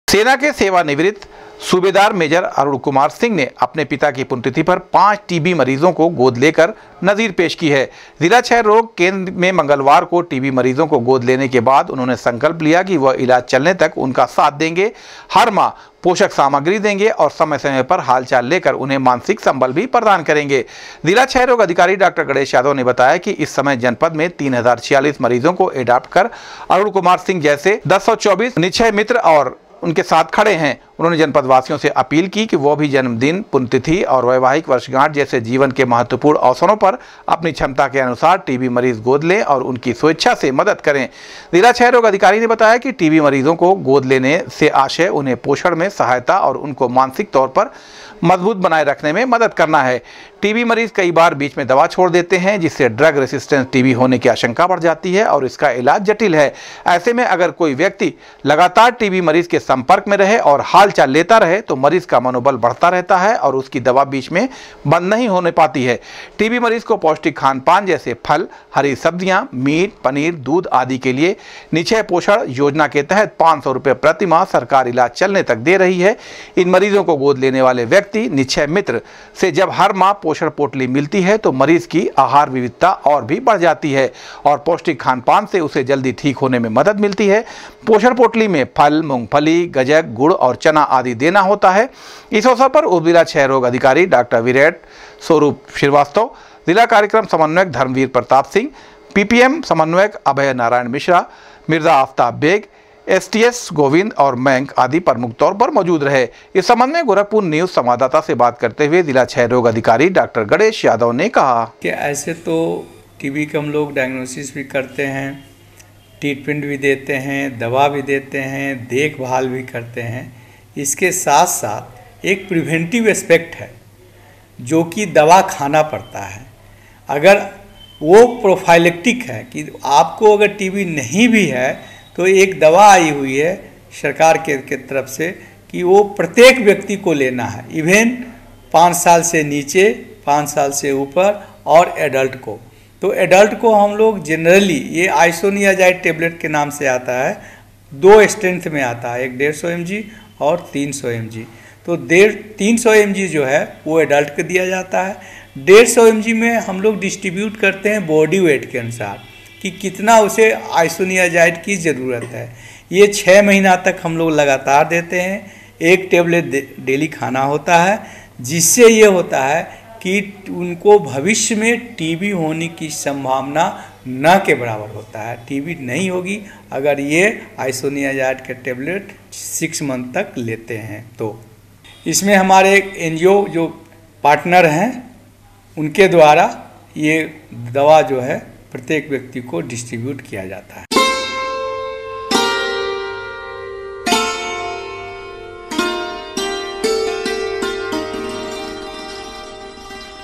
सेवानिवृत्त सूबेदार मेजर ने पिता की पुण्यतिथि पर पांच टीबी मरीजों को लिया गोद
सेवानिवृत्त सूबेदार मेजर ने पिता की पुण्यतिथि पर पांच टीबी मरीजों को लिया गोद, इलाज चलने तक मरीजों की पोषक सामग्री के साथ देते रहेंगे मानसिक संबल